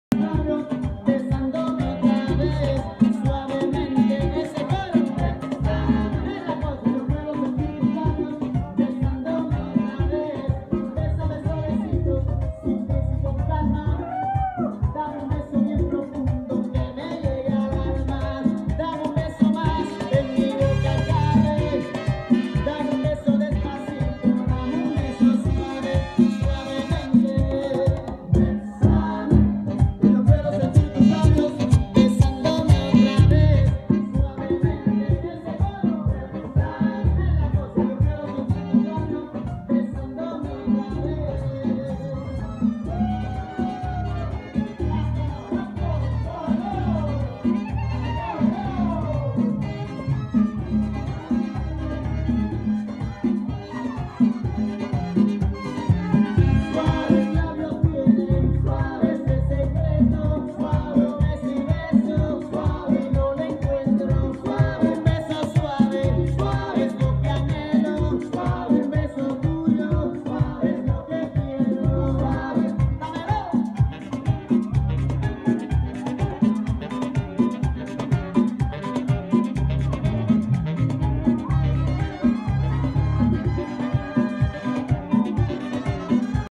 DJ Matt entertains a crowd - Latin Wedding in Orlando
DJ Matt entertains a Latin wedding crowd at Ceviche's Tappas in Orlando, FL!